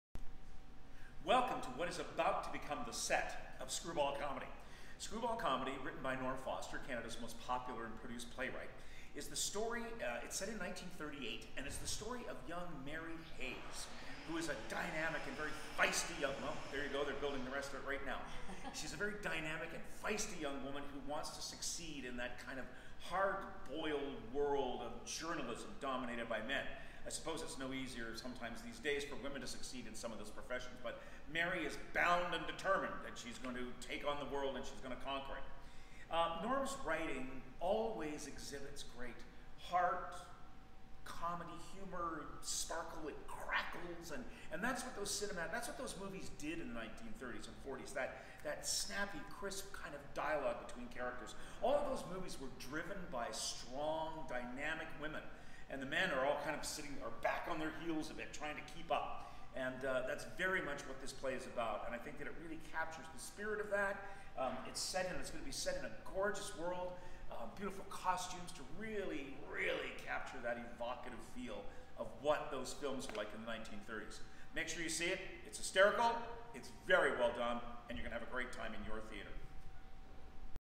David Nairn on Screwball Comedy by Norm Foster
David Nairn Artistic Director of Theatre Orangeville gives you the inside scoop on Screwball Comedy by Norm Foster. Running from April 5-22